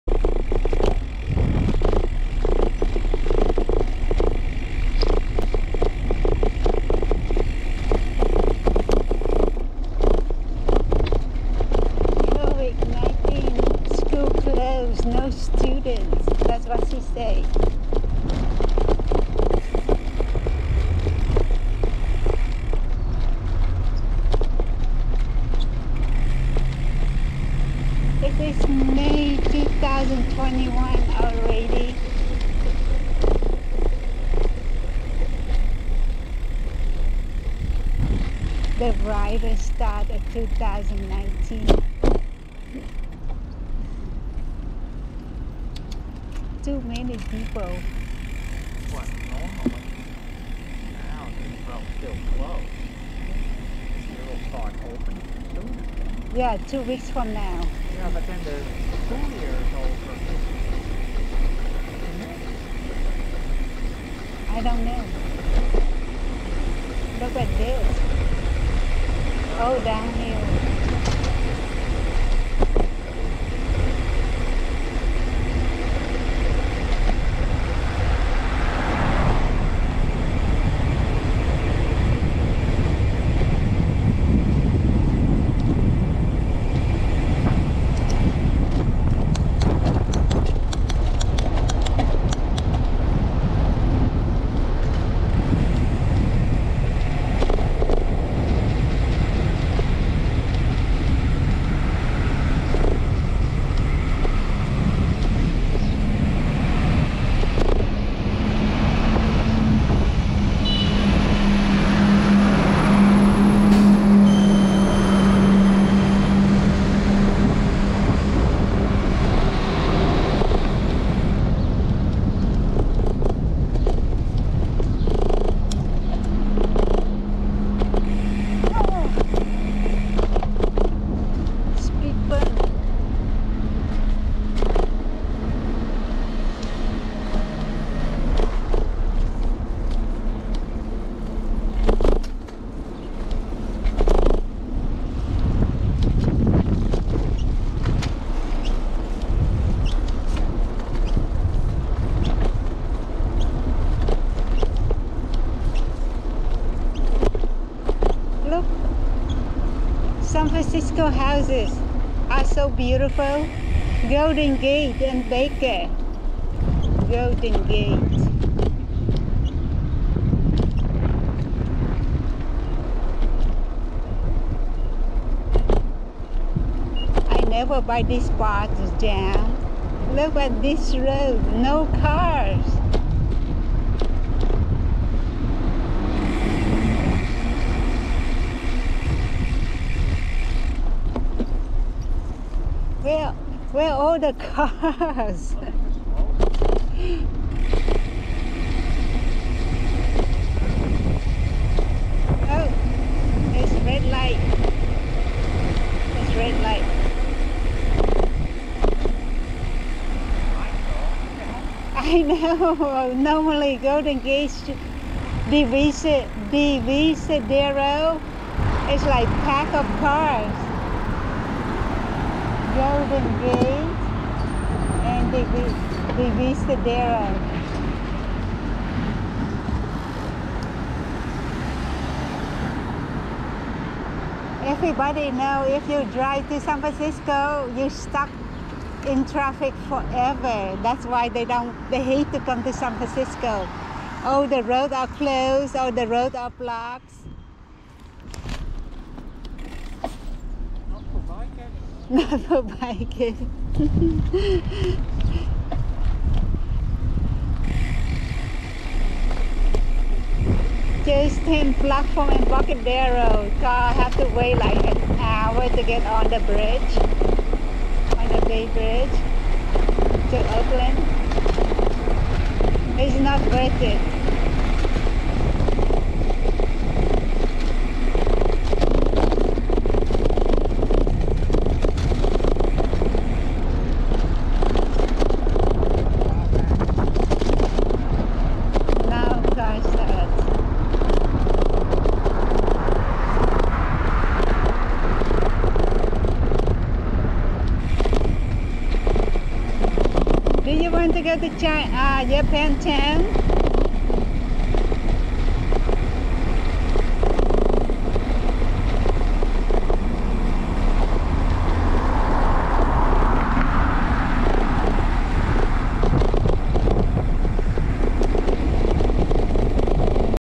StreetTourSF: University of San Francisco and Golden Gate Avenue San Francisco
My StreetTourSF cycling rides and BayTrail rides.
See my profile for links to my Bay Trail map and StreetTour map.